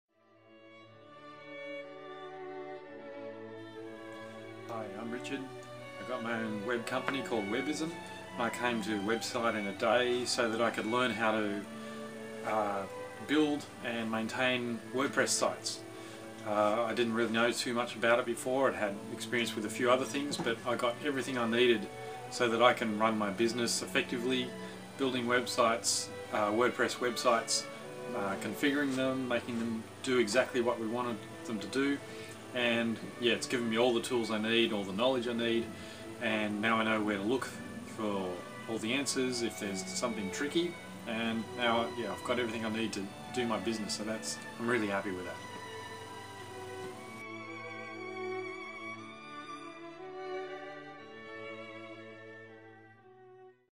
Website In a Day - WordPress Training for Webbism
- In a Day recently conducted a customised Website In a Day Training/Consulting course in Brisbane, for Richard from Webbism to give a better understranding on how he can get the most out of WordPress to create websites for his own clients.